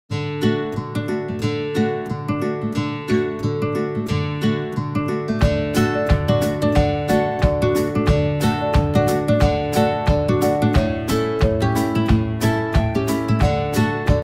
Siberian Husky Puppy Licking A Lemon.
Another test video of my puppy!